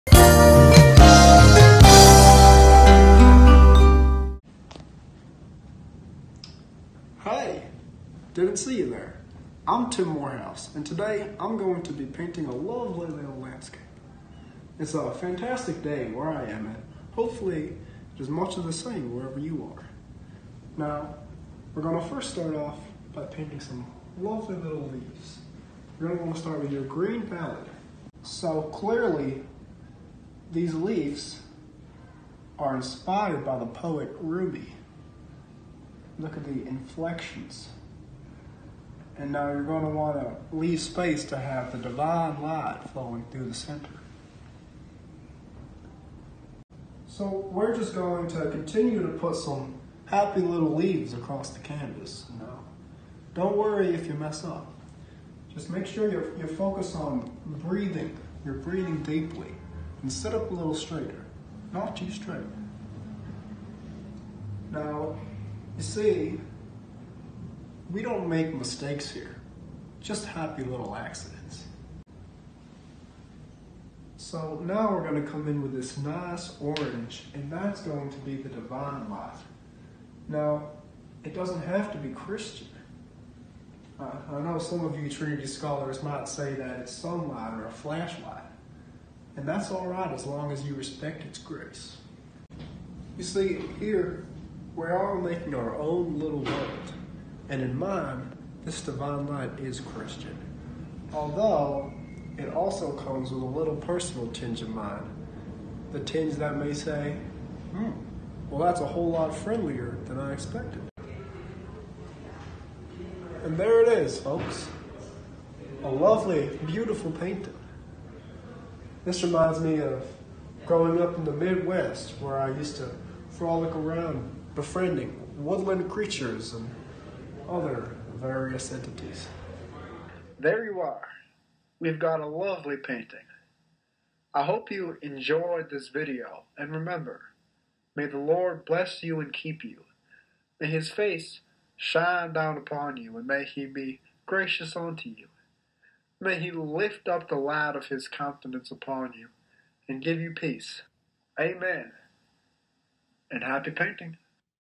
[Cut for Time] Senior Skits 2018: The Joy of Painting with Tim Morehouse
Trinity School Halloween Skits—Class of 2019
10/30/18

Tim Morehouse draws some nice little clouds in a meditation of painting.